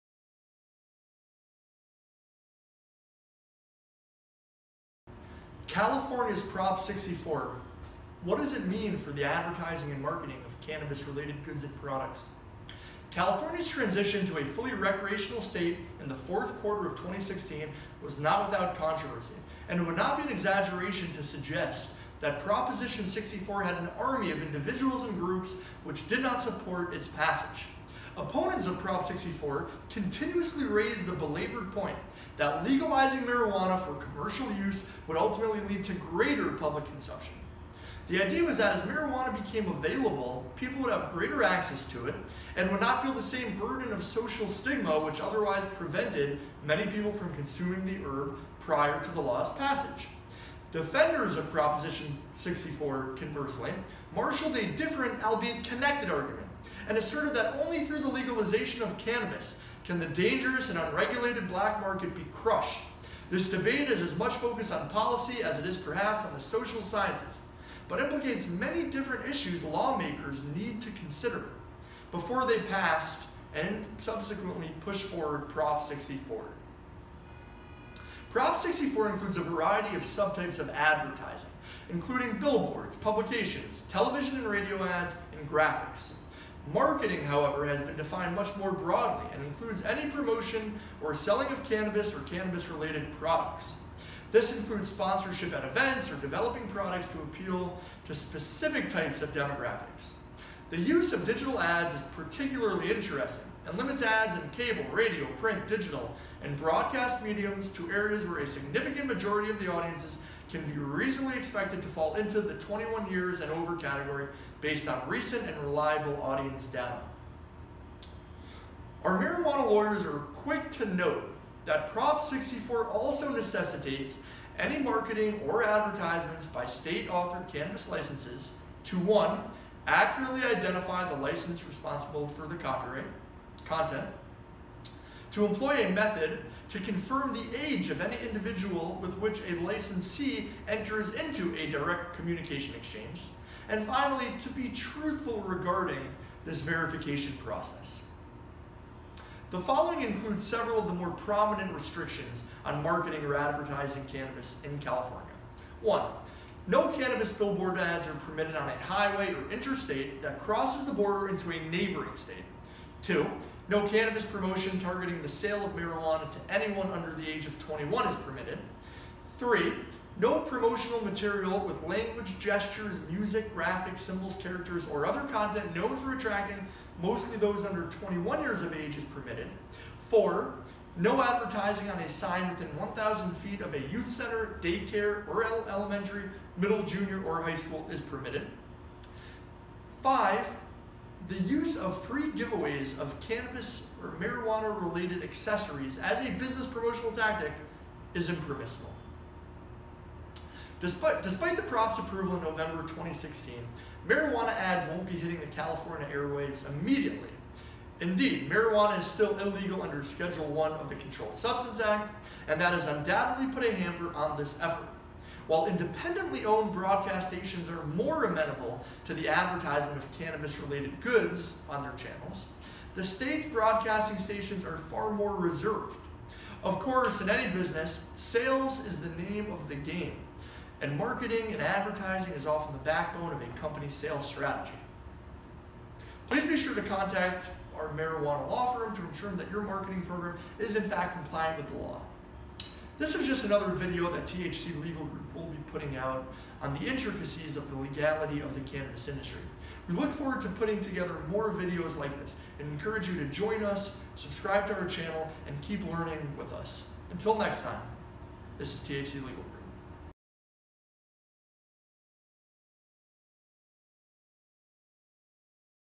Marijuana Advertising Laws in California - Prop 64
California's Prop 64 includes a variety of subtypes of “advertising” including billboards, publications, television and radio ads, and graphic(s). Marketing, however, has been defined much more broadly and includes any promotion or selling of cannabis or cannabis-related products. This includes sponsorship at events or developing products to appeal specific types of demographics.

The use of digital adds is particularly interesting and limits ads in cable, radio, print, digital, and broadcast mediums to areas where a significant majority of the audience can be reasonably expected to fall into the 21 years and over category based on recent and reliable audience data.